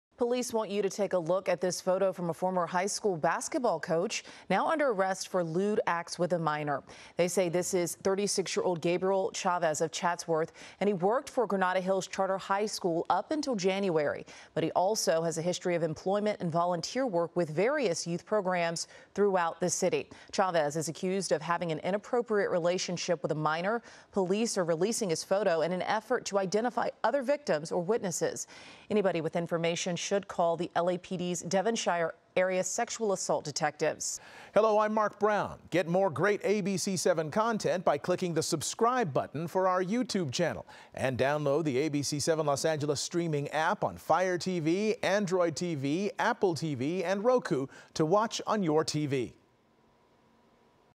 Former LA high school basketball coach arrested for inappropriate relationship with a minor
A former Granada Hills high school basketball coach and youth programs volunteer has been arrested for lewd acts with a minor. MORE